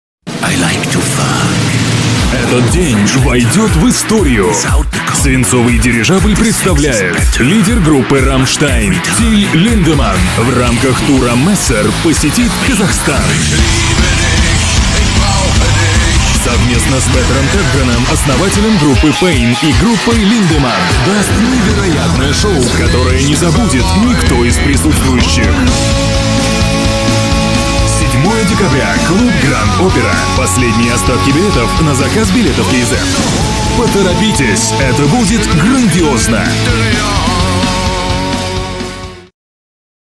Till Lindemann in Almaty
Промо ролика тура Тиля Линдеманна в Алматы